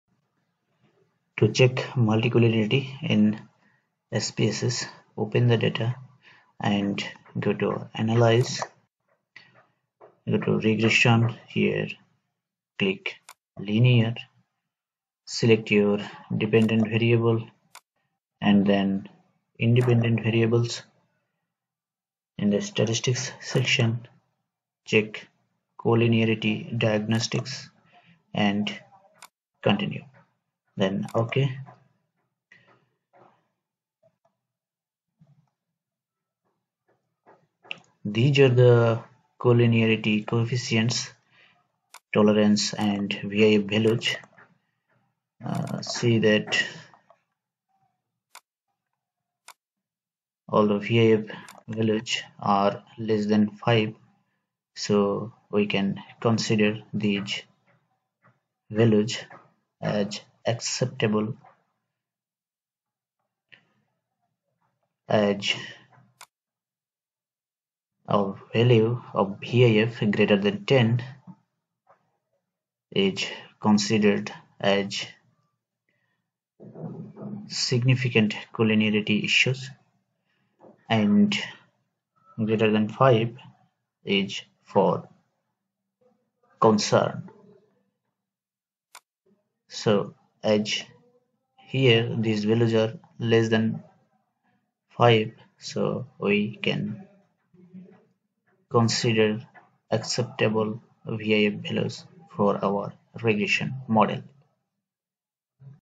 Check Multicollinearity in SPSS | Multiple Linear Regression
How to check multicollinearity for regression model in IBM SPSS statistics.